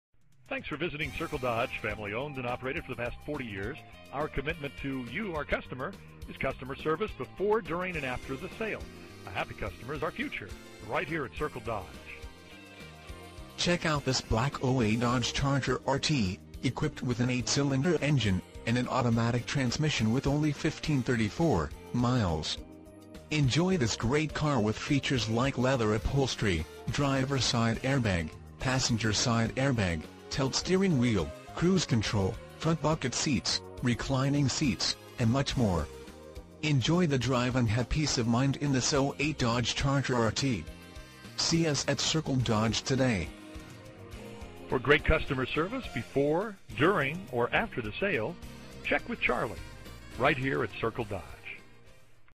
SOLD - 2008 Dodge Charger R/T 08723 Circle Dodge
Circle Dodge

781 State Hwy 70

2008 Dodge Charger R/T

  1,534 Miles

VIN - 2B3LA53H98H286688

You will love this Black 2008 Dodge Charger R/T, equipped with an 8 Cyl. engine  and an automatic transmission with  only 1,534 miles. Enjoy this great car with features like Leather Upholstery, Driver Side Air Bag, Passenger Side Air Bag, Tilt Steering Wheel, Cruise Control, Front Bucket Seats, Reclining Seats, and much more. Enjoy the drive and have peace of mind in this 2008 Dodge Charger R/T. See us at Circle Dodge today!